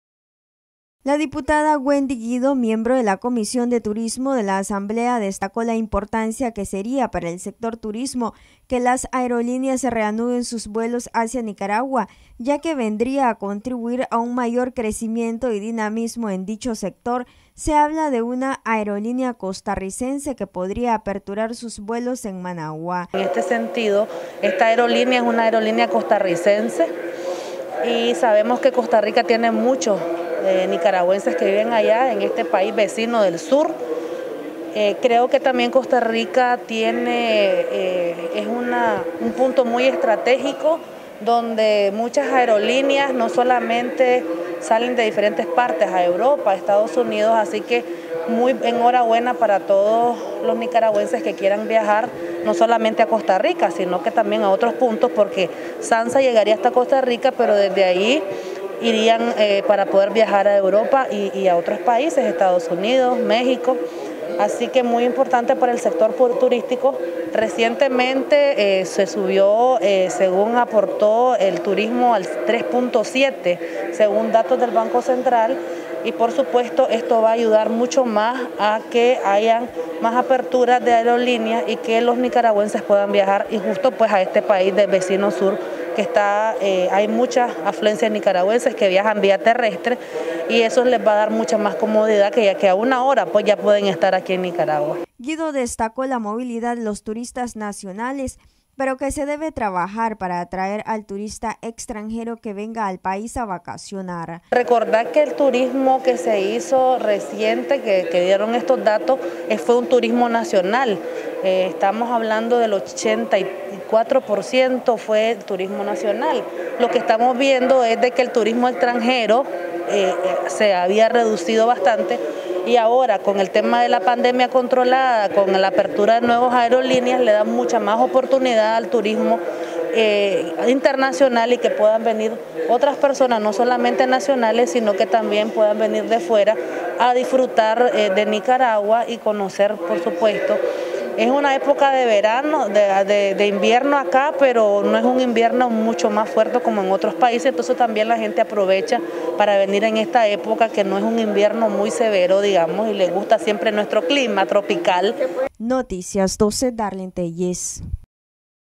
Reanudación de vuelos vendría oxigenar al sector turismo
Noticias12Nicaragua A pesar que el sector turismo ha venido recuperándose, es de mucha importancia que se reanuden los vuelos, ya que se dinamizaría dicho sector afirma Wendy Guido, quien valoró de positivo para el sector si se llegase aperturar aerolínea costarricense hacia Nicaragua.